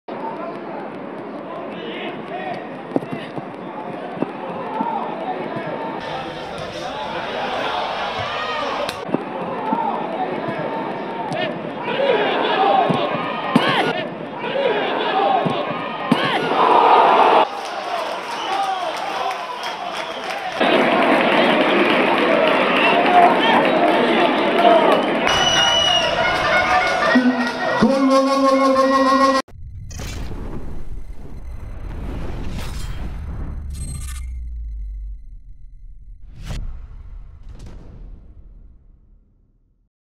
EL GOLAZO DE JORDI SÁNCHEZ CON DEDICATORIA A CHEMA SANZ
Jordi Sánchez, delantero del VCF Mestalla, anotó un gran gol ante el CD Castellón y se lo dedicó a Chema Sanz, el entrenador del filial valencianista.

Jordi Sánchez, VCF Mestalla striker, scored a great goal against CD Castellón and dedicated it to his coach Chema Sanz